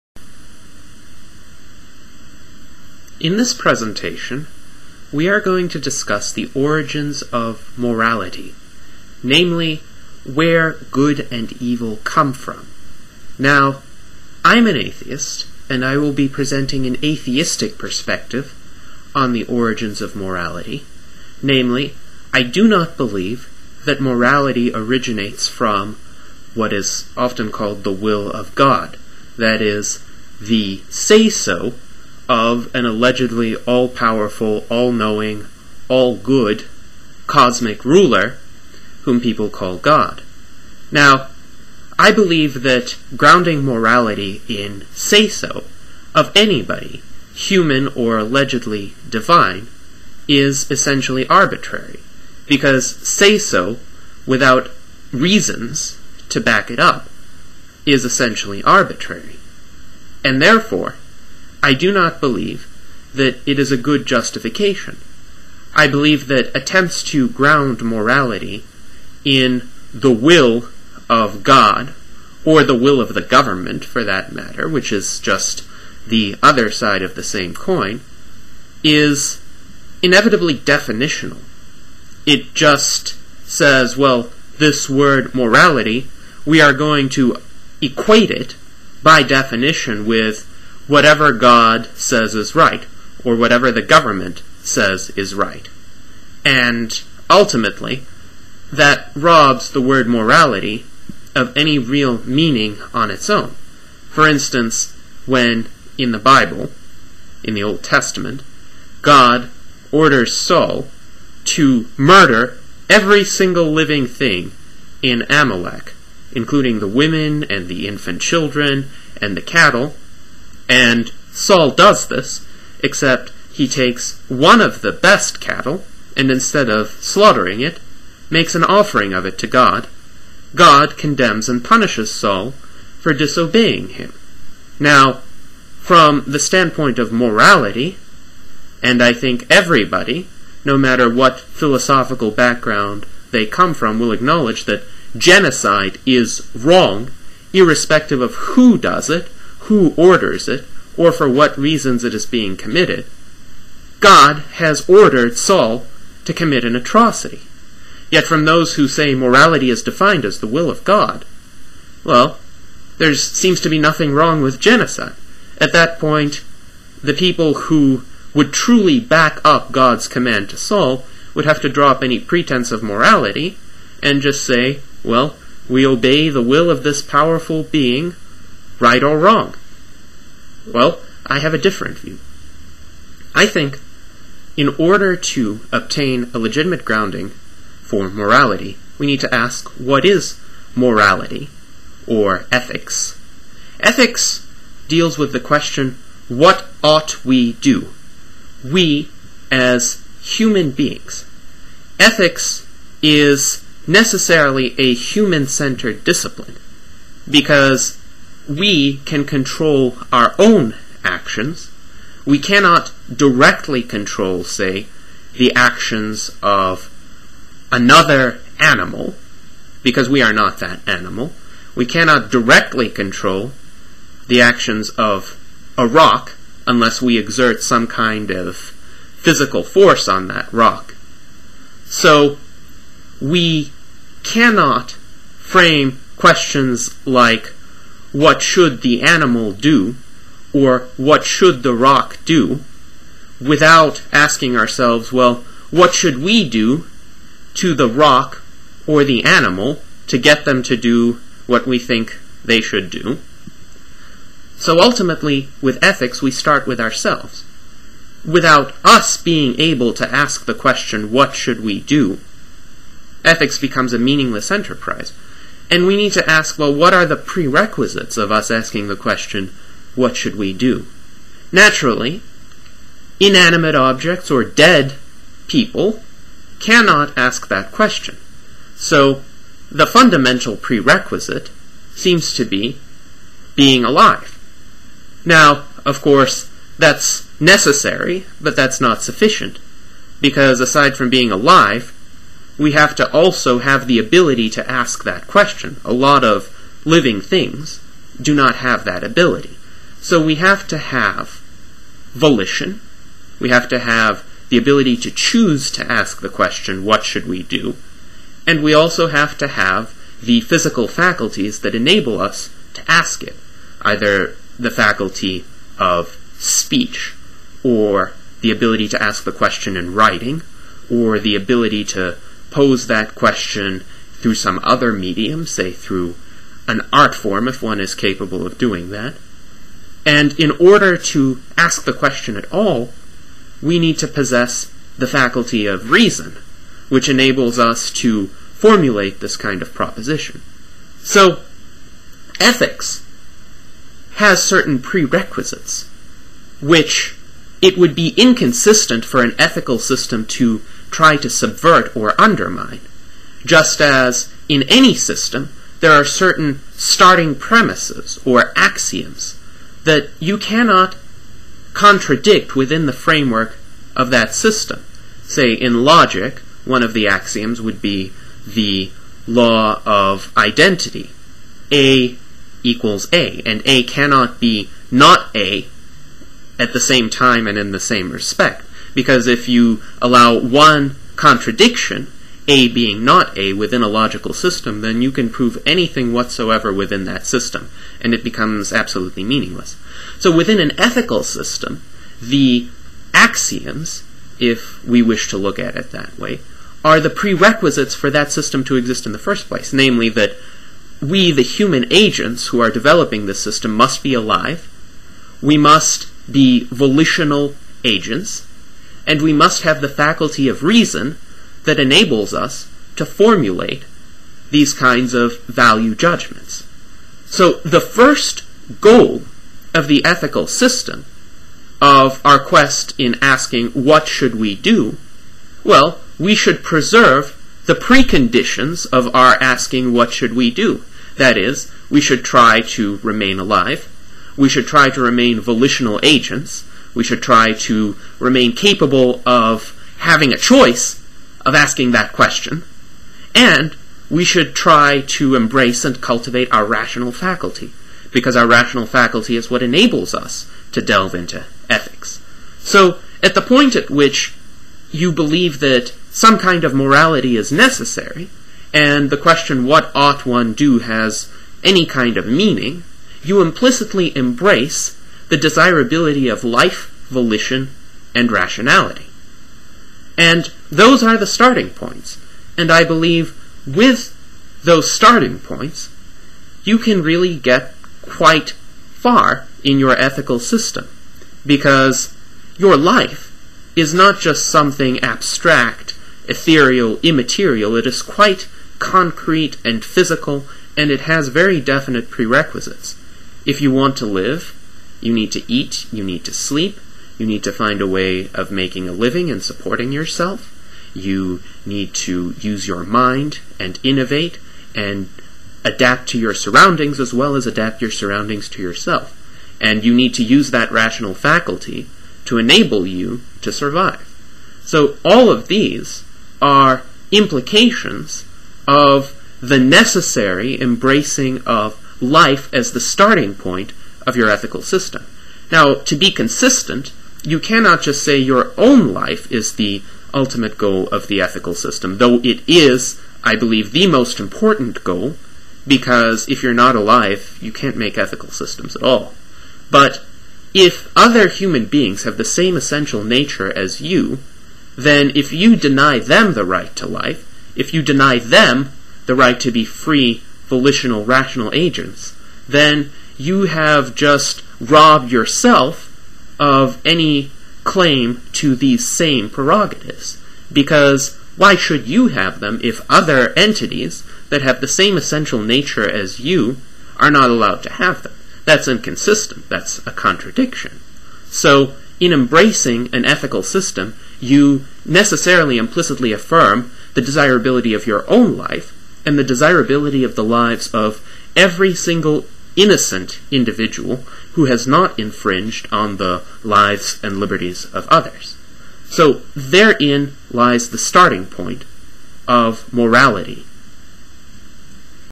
Life as the Origin and Basis of Morality - Part 1 of 2
Part 1 of this video presentation explains why any rationally grounded, consistent ethical system presupposes the desirability of human life, the human volitional faculty, and rationality.